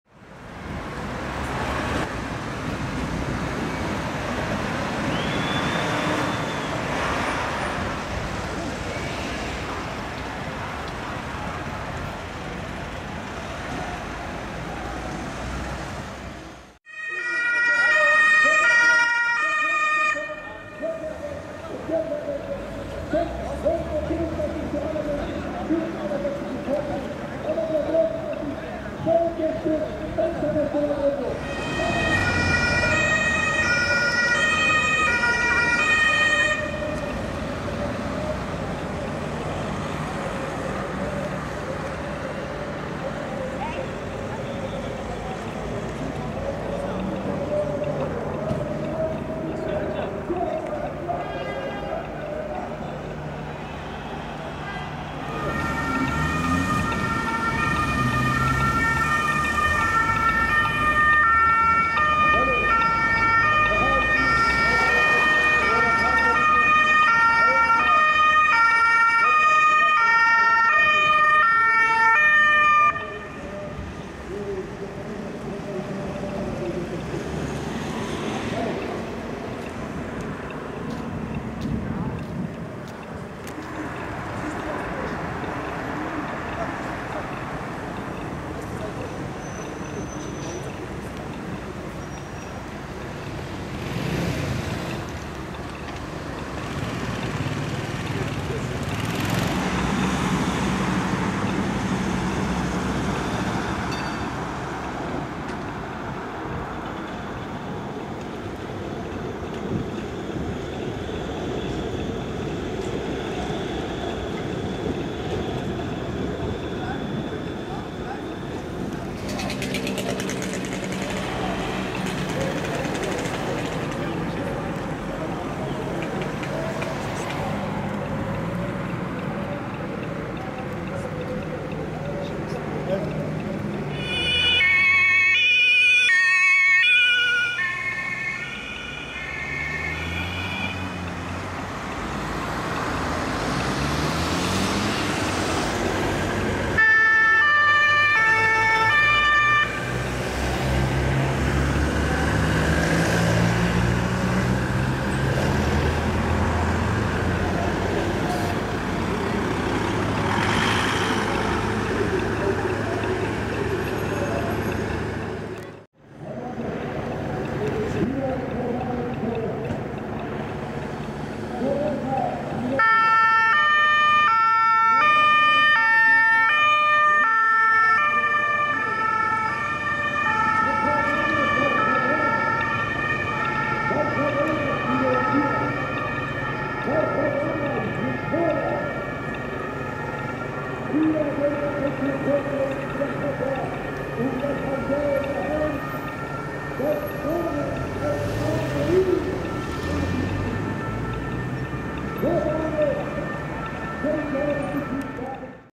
Kurdendemo - Großeinsatz Polizei, Rettungsdienst und Feuerwehr Düsseldorf
Einsatzfahrzeuge der Feuerwehr Düsseldorf (BF und FF) sowie diverse Polizeieinheiten auf Einsatzfahrt während einer Kurdendemo am Landtag in Düsseldorf. Zu sehen sind:

1. ELW 1 FF Düsseldorf TeKomm-Zug (Florian Düsseldorf 20 ELW1 01)
2. RTW BF Düsseldorf FW 1 (Florian Düsseldorf 01 RTW 02)
3. NEF BF Düsseldorf FW 1 (Florian Düsseldorf 01 NEF 01)
4. RTW BF Düsseldorf FW 3 (Florian Düsseldorf 03 RTW 03/04)
5. LauKW Polizei NRW 2. TEE Wuppertal
6. FuStW Polizei NRW PP Düsseldorf (Autobahnpolizei Düsseldorf)
7. HGrKW Polizei NRW 3. BPH Dortmund
8. 3x HGrKW Polizei NRW 3. TEE Köln (darunter 2 ehemalige FüKW)
9. RTW BF Düsseldorf FW 3 (Florian Düsseldorf 03 RTW 03/04) //

Düsseldorf, Nordrhein-Westfalen, Deutschland - 10.2014